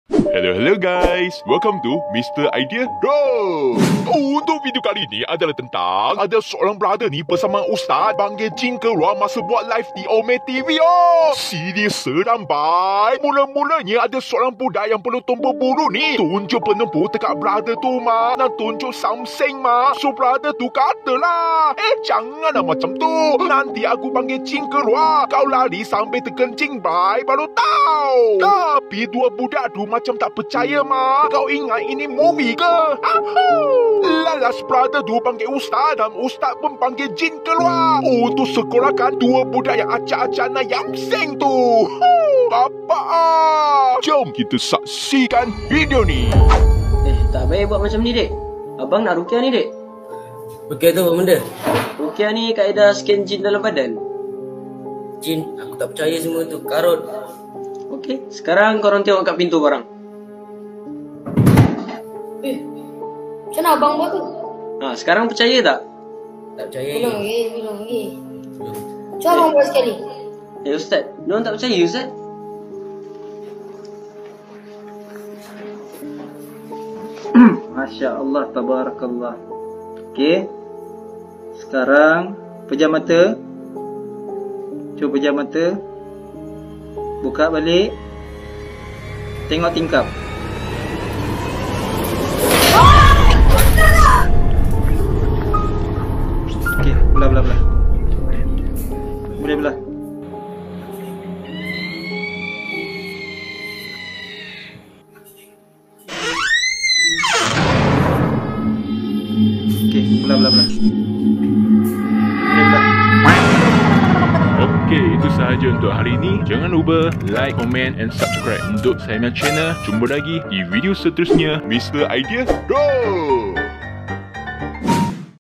USTAZ PANGGIL JIN KELUAR Untuk Sekolahkan 2 Member Hingusan | TUNJUK PENUMBUK Masa Buat Live 0METV!!
Kisah Seram Uztas Panggil Jin Keluar!!! Hantu Malaysia Online!!!
🔴Yang Berminat Nak Beli Baju 'RAKYAT JAGA RAKYAT'😊😊😊:
*Nantikan Video MR. IDEA DOH seterusnya🤓🤓🤓